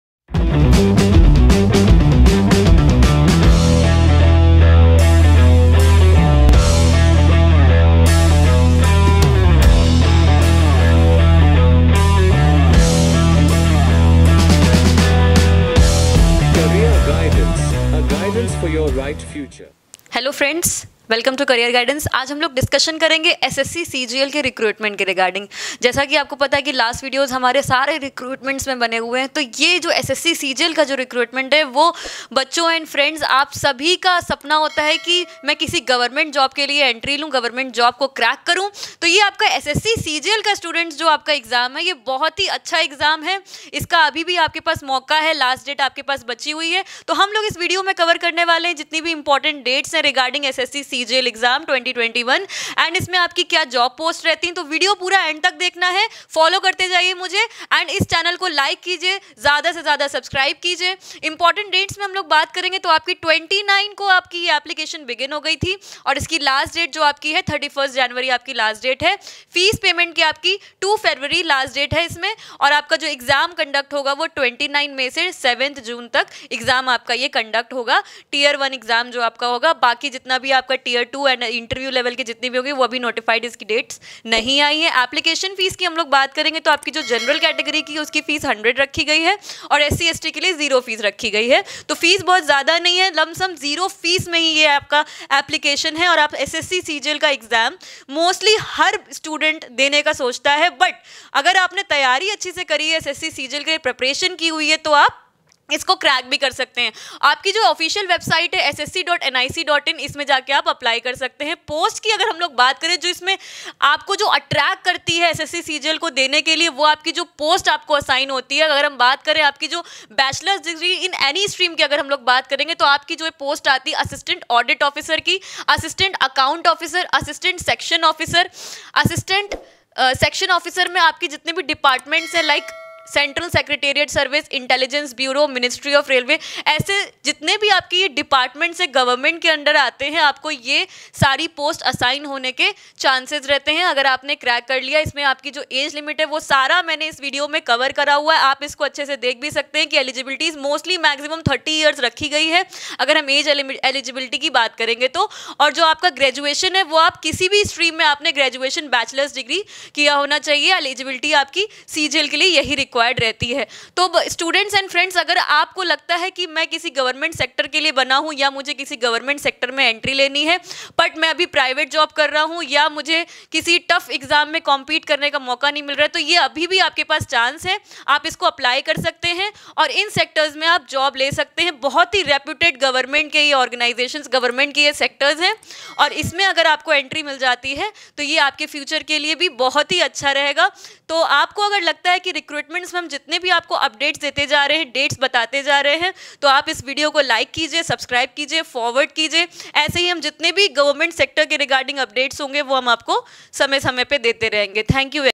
SSC CGL 2021 | एप्लिकेशन की last Date नज़दीक, ग्रेजुएट्स न चूकें मौका | Exam Syllabus, Eligibility.
The official notification of SSC CGL 2021 is officially released on 29th December 2020. The Online Application is available from December 29, 2020, to January 31, 2021, open for one month from the date of the start date of the online application.
The exam dates for SSC CGL 2021 has been released by SSC along with its official notification. Check SSC CGL 2021 exam date.
The tentative vacancies are released with the notification. The tentative vacancies as released by the SSC are 6506 (Group ‘B’ Gazetted-250, Group ‘B’ Non-Gazetted-3513, Group ‘C’-2743). You can check the previous year’s vacancies from the table below. The Vacancy for SSC CGL 2019-20 exam has been released. For 2019-20, there are a total of  8582 Vacancies out of which 3674 vacancies are for the General category of candidates. There has been a decrease in the no. of vacancies as compared to SSC CGL 2018-19. Check the SSC CGL Vacancy Comparision Year wise to get an idea of the trend in the vacancy.